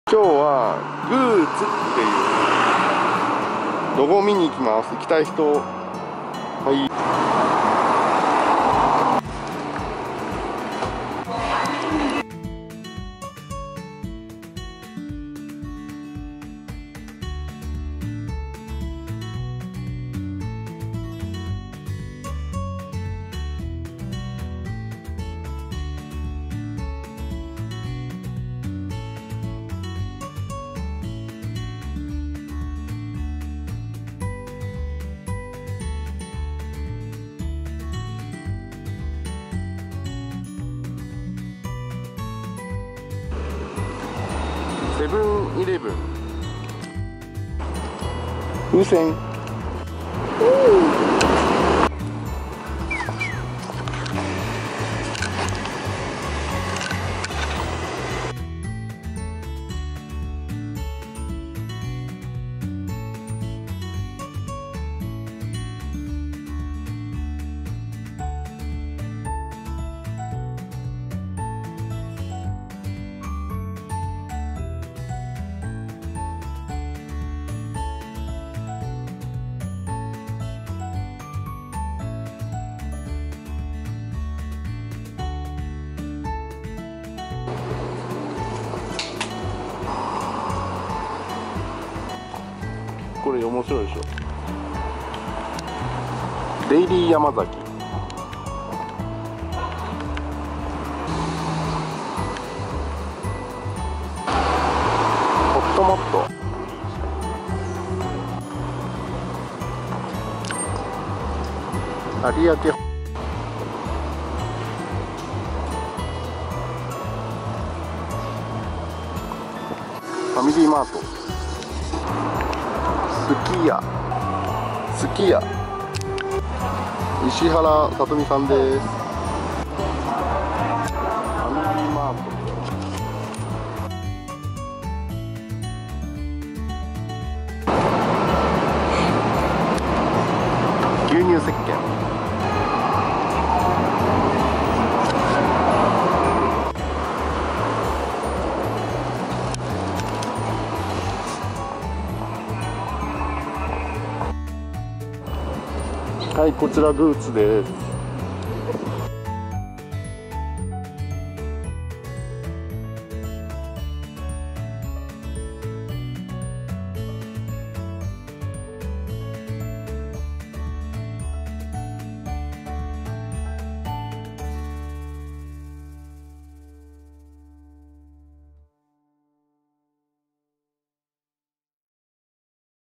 企業ロゴを探す！22 ロゴツアーズ gooz（グーツ）・デイリーヤマザキ・ローソン＋スリーエフのコンビニロゴを見に行く！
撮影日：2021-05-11
撮影地：神奈川県横浜市中区
撮影・編集：なかC
■キッズコネクション
チャプター：
0:00​​​​​​​​​​​​​​​​​​｜オープニング
0:06​｜花夏彩
0:09｜市営地下鉄 関内駅
0:30｜関内駅周辺の街
0:42｜ナチュラルローソン
0:44｜セブン-イレブン
0:48｜ディズニーの小さいアイテム
0:52｜避難器具の案内板
0:55｜ローソン・まちカフェ
0:59｜スルガ銀行
1:13​｜セブン-イレブン
1:30​｜レンガ造りの花壇
1:33​｜デイリーヤマザキ
1:41​｜文房具屋楽文堂
1:44​｜ほっともっと
1:46​｜サン理容室
1:48｜ローソン＋スリーエフ（エル・ティーエフ）
1:51​｜ありあけ（横濱菓楼ハーバーズムーンありあけ本館）
1:54​｜横浜DeNAベイスターズ
2:00｜ファミリーマート
2:02｜すき家
2:07​｜石原さとみ
2:10​｜ファミリーマート
2:18​｜B-stop®（広告付バス停）
2:20​｜牛乳石鹸
2:26​｜ボタニスト（アイエヌイー）・スパトリートメント（ウェーブコーポレーション）
2:34​｜gooz（グーツ、店内ファーストフードを強化したスリーエフのコンビニエンスストア）
2:42｜エンディング